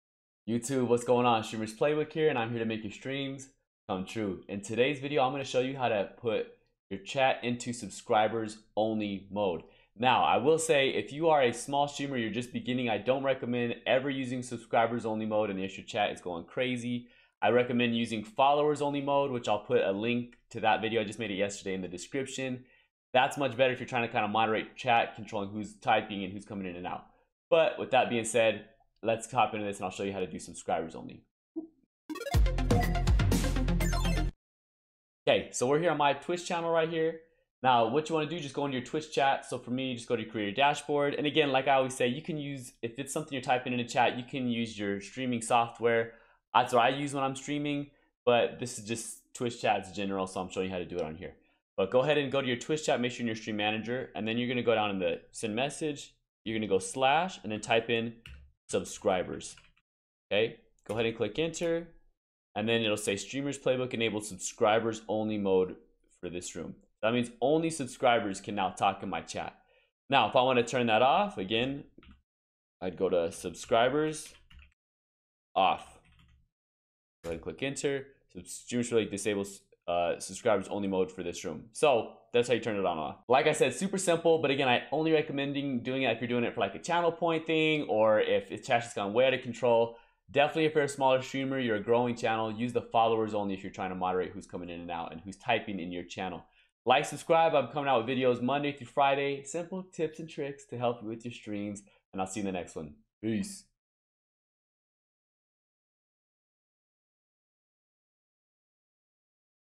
How To Enable Subscribers Only Mode On Twitch
In this video I will show you how to enable subscribers only chat. Easy and simple tip to make your streams come true!